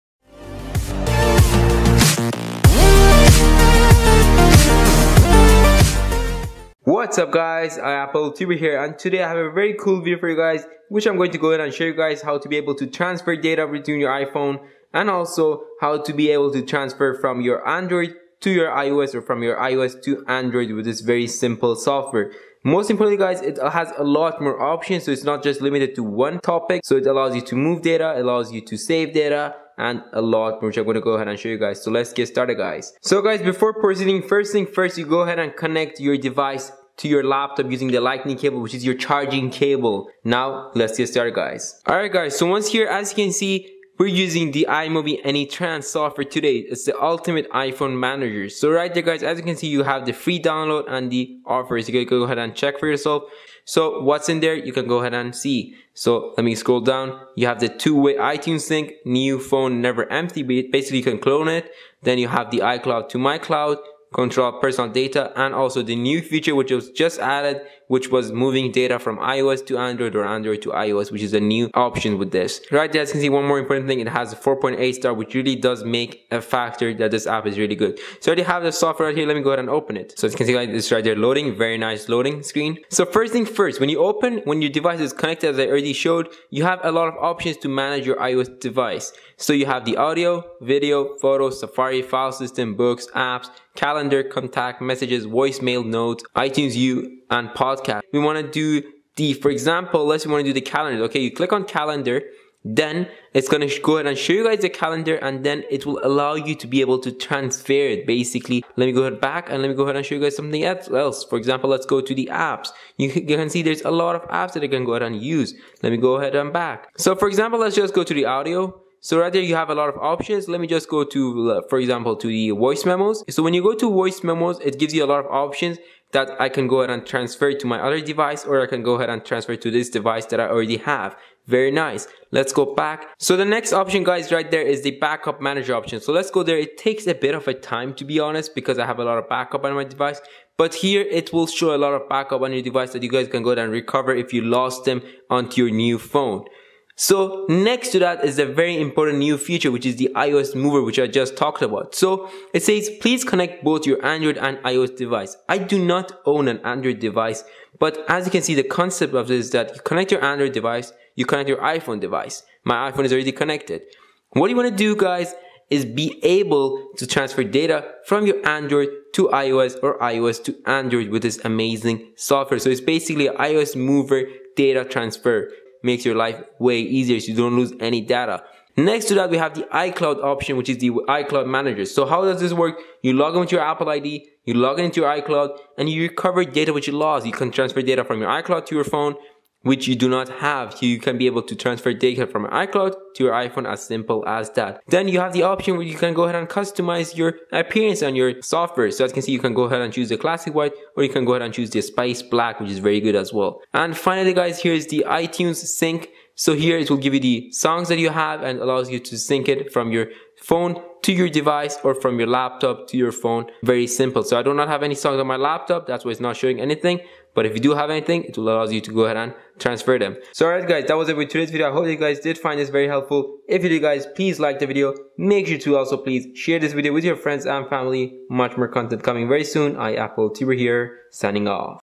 How to Transfer Files, Data, Manage Music, Photos, Save iOS Backups and more with One Click!
How to Transfer Files, Move Data from Android to iOS, Manage Music, Photos, Save iOS Backups and more with One Click!